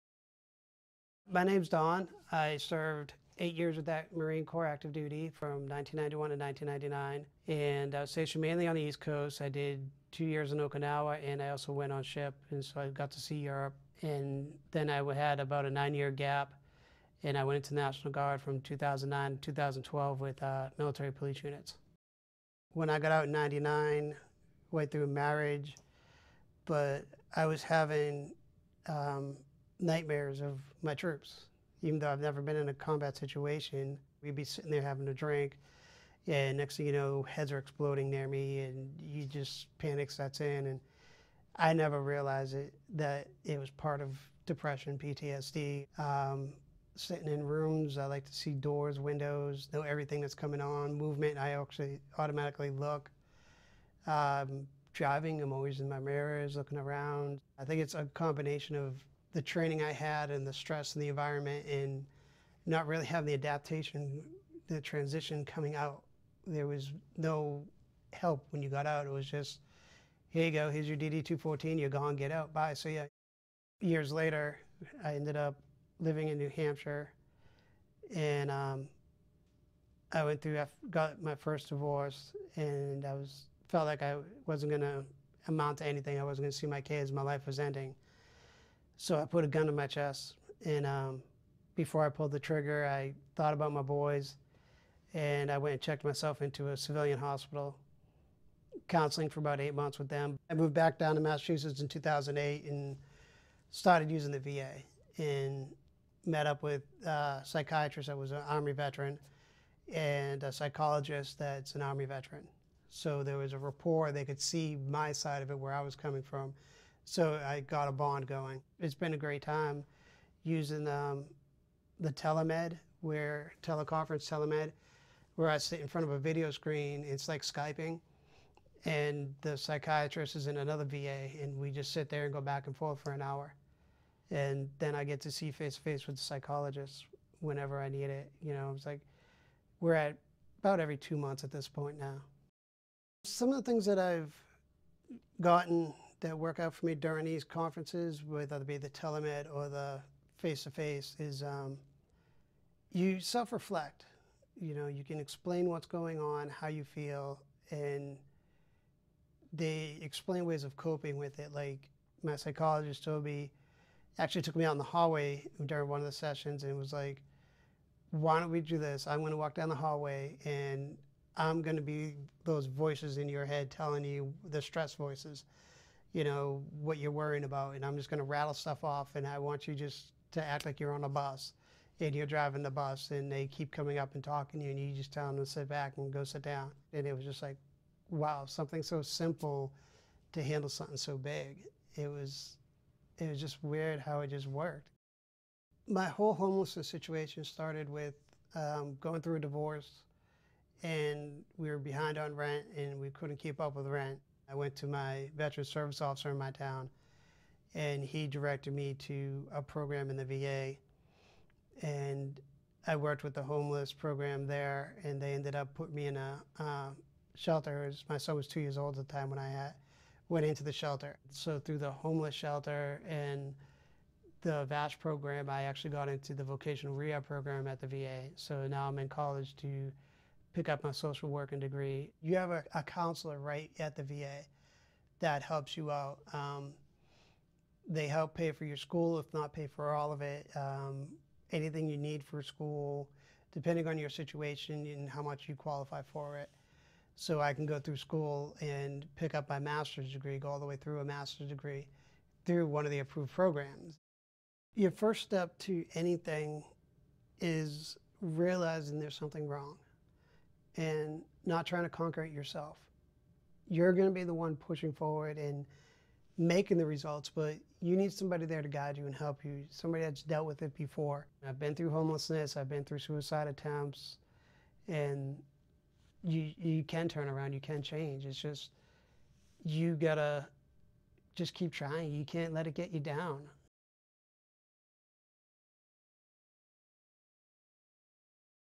Finding support through therapy and telemedicine
After Don left the Marines, he had nightmares and felt on edge. Following his divorce, he felt hopeless. Don decided to reach out to VA and connected with a psychologist. He also met with a psychiatrist through VA Telehealth Services. By taking advantage of programs offered to Veterans, Don was able to move forward with his life.